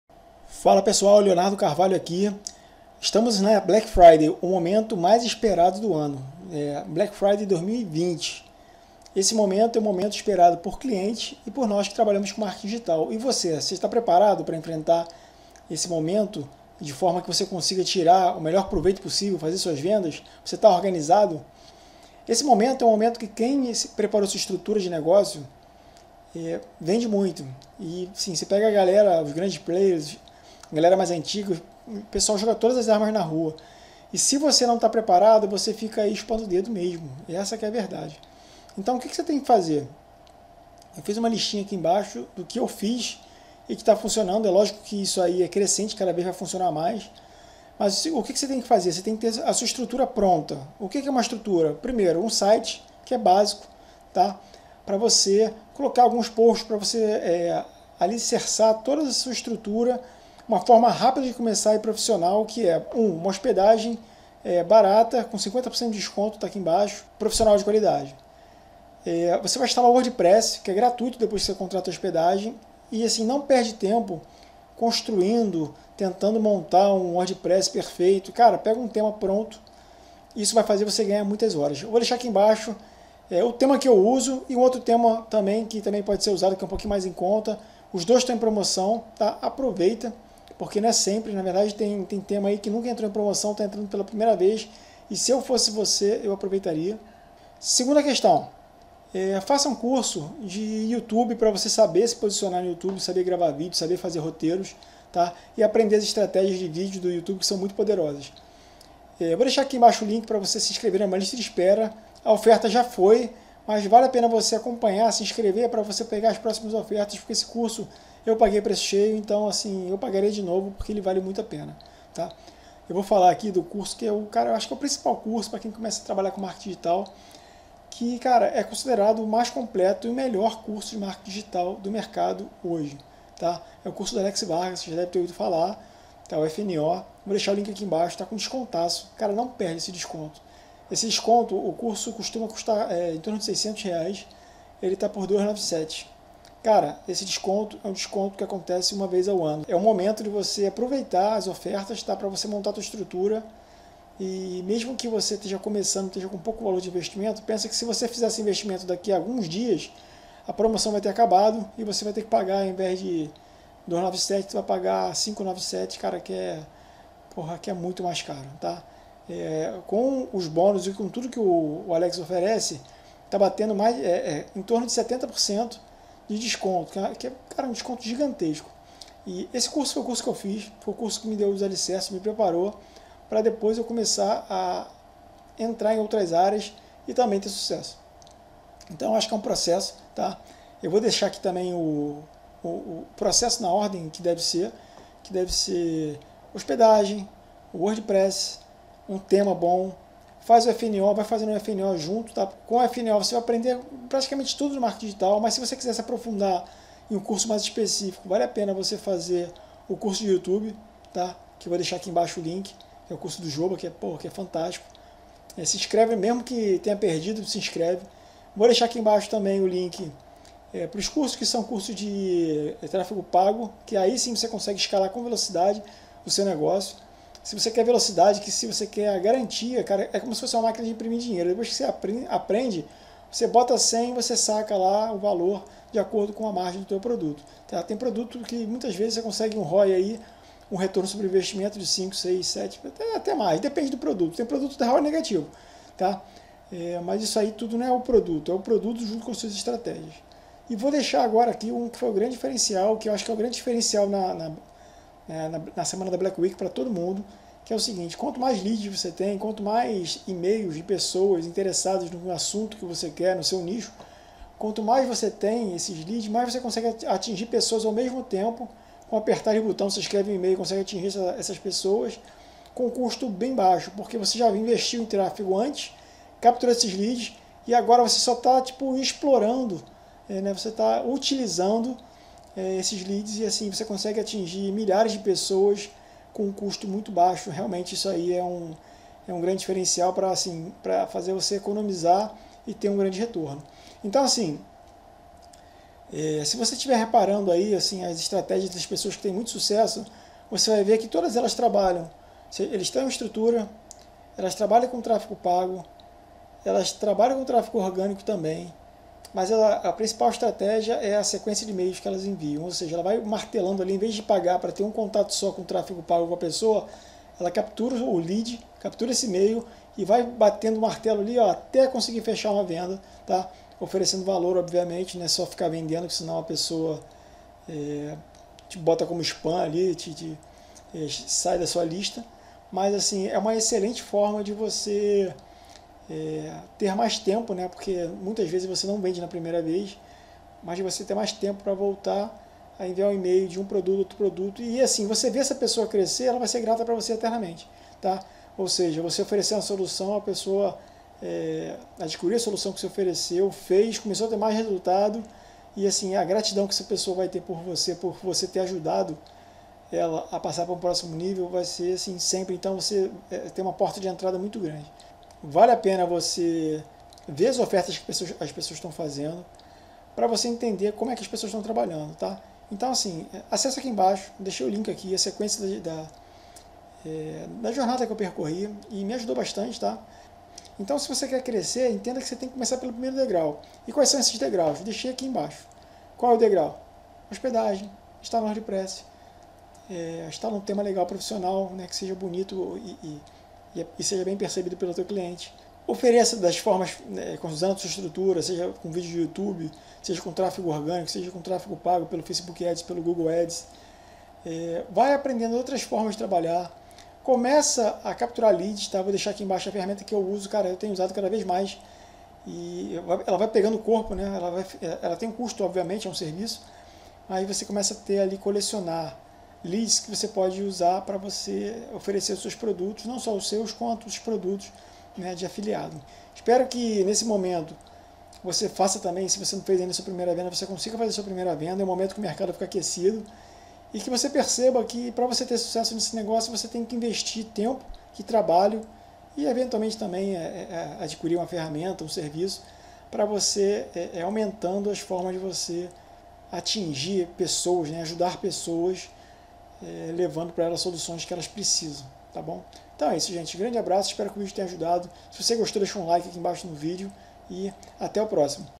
Black Friday 2020: Criar um Negócio Digital Vender na Hotmart e Ganhar Dinheiro na Internet Afiliado
✅ CURSO GRÁTIS DE MARKETING DIGITAL

✅ Plugin WP RGPD PRO para estar dentro da Lei LGPD

Nesse Vídeo eu Mostro Como Criar um Negócio Digital e Vender na Hotmart ou Monetizze e Ganhar Dinheiro na Internet com Marketing Digital como Afiliados ou Produtor.

Revelei a jornada que percorri  e que me legou da primeira venda que nunca vinha, até montar minha estrutura de negócio e começar a fazer minhas vendas diariamente usando ferramentas que vão acelerar o seu resultado e fazer começar a viver de marketing digital.